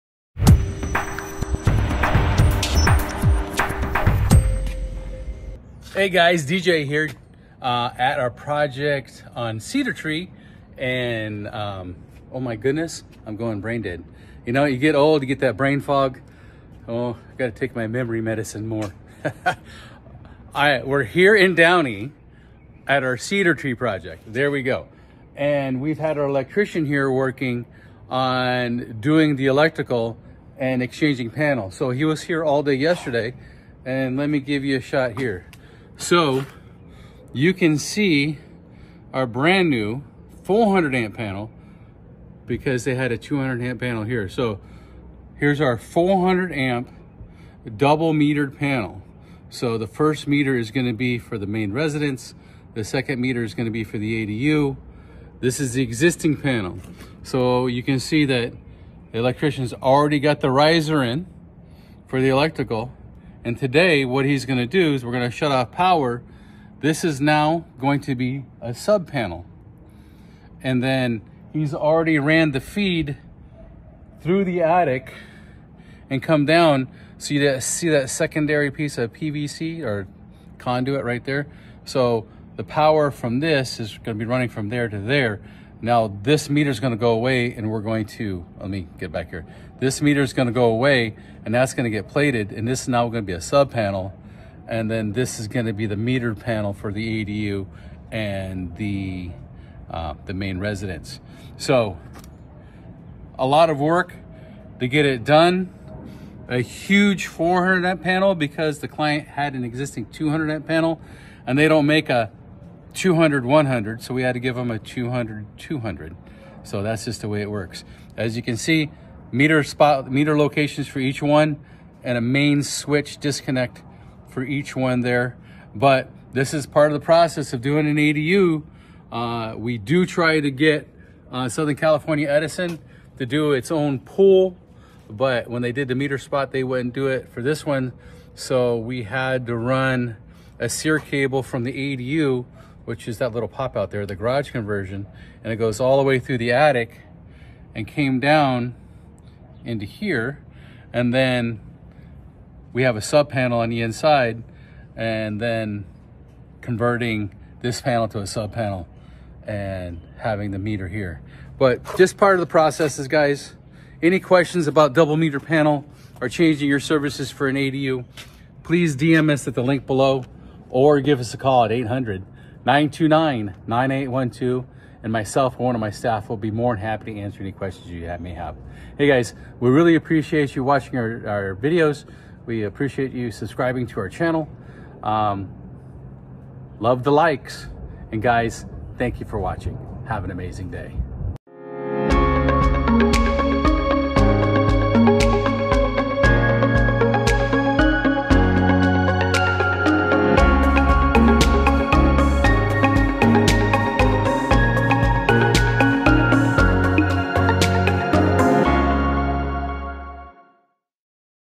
"ADU Cedartree Rd. Downey | Part 16: ADU Double Electrical Meter Installation: Step-by-Step Guide 🏠⚡
Explore the latest installment of our ADU Cedartree Rd. Downey series in Part 16: ADU Double Electrical Meter Installation: Step-by-Step Guide. 🏠⚡

Join us as we provide a detailed walkthrough of this crucial process, ensuring seamless integration of electrical systems.

DREC Construction Services, Inc.